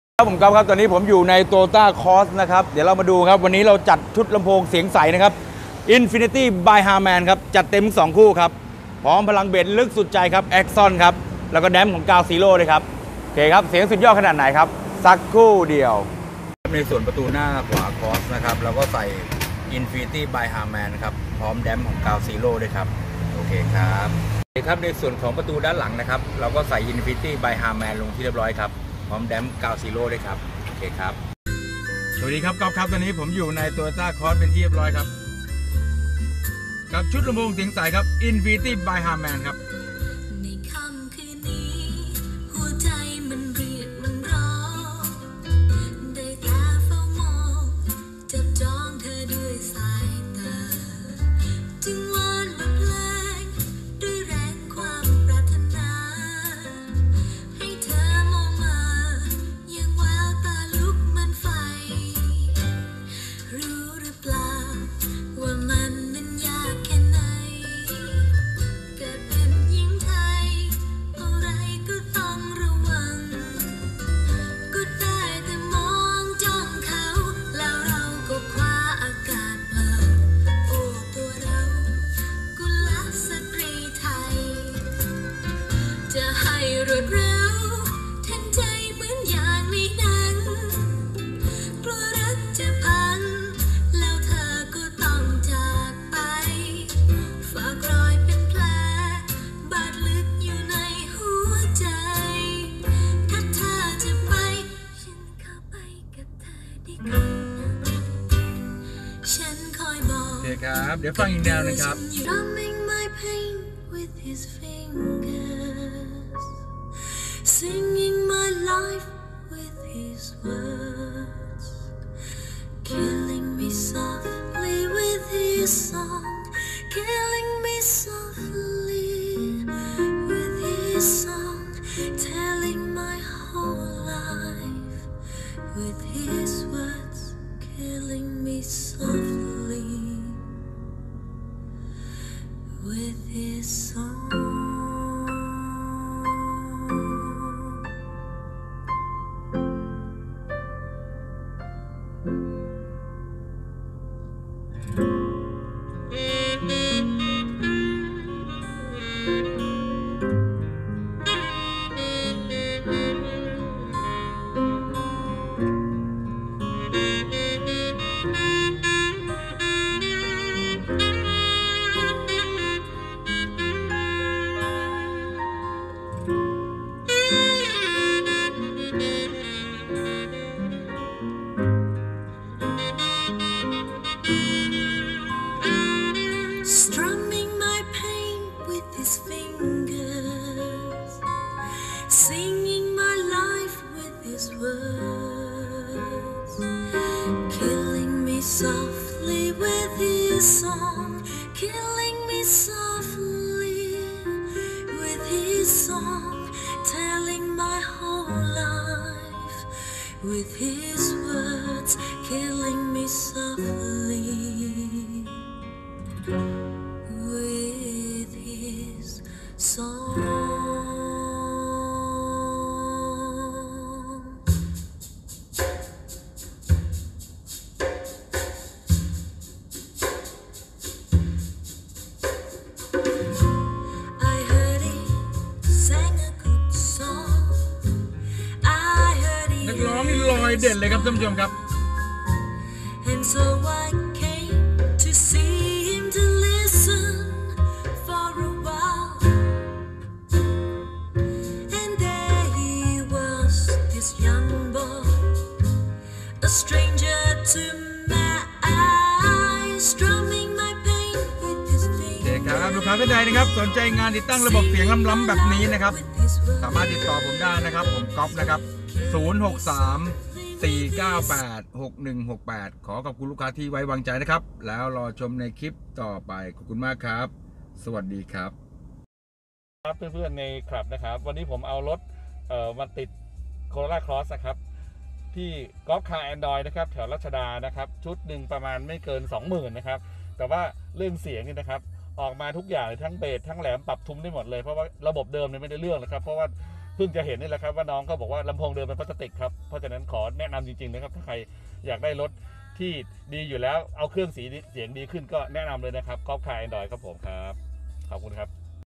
ชุดอัพเกรดลำโพงเสียงใส Infinity by Harman TOYOTA Cross
ชุดอัพเกรดลำโพงเสียงใส
Infinity by Harman +Sub box aczon +Damp GROUND Zero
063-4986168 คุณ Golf
Line veerapat8899

มีบริการติดตั้งนอกสถานที่
มีบริการรับ -ส่ง ด้วยครับ